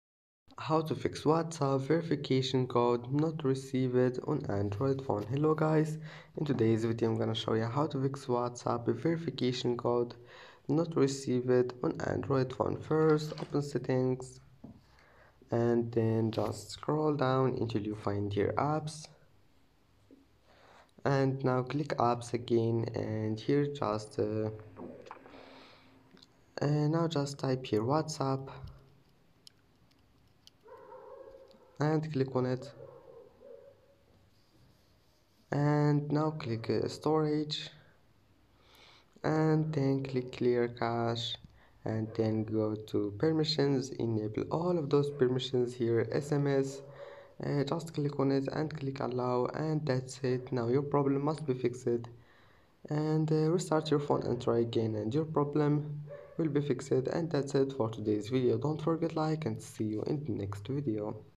How to Fix and Solve Whatsapp Verification Code Not Received on Any Android Phone 2022
in this video we discuss :